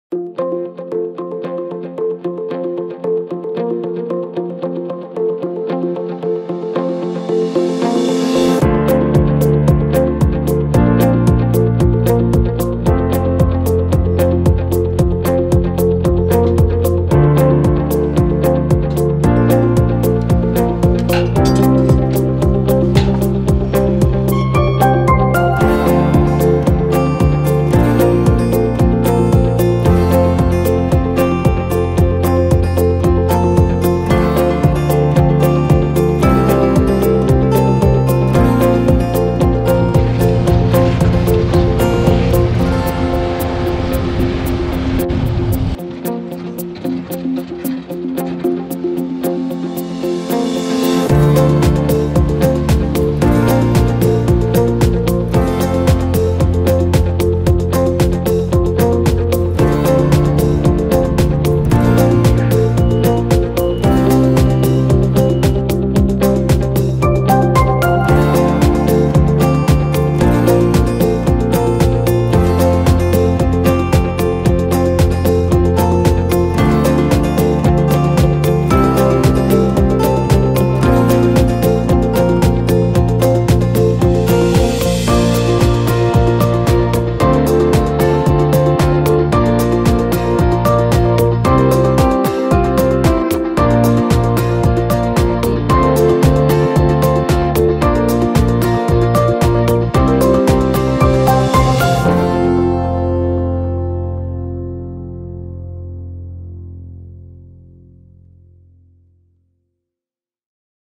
The construction site in spring 2020
In May 2020 preparatory work will continue at the Hanhikivi 1 nuclear power plant construction site. For example reinforcement workshop and turbine equipment storage area are being built.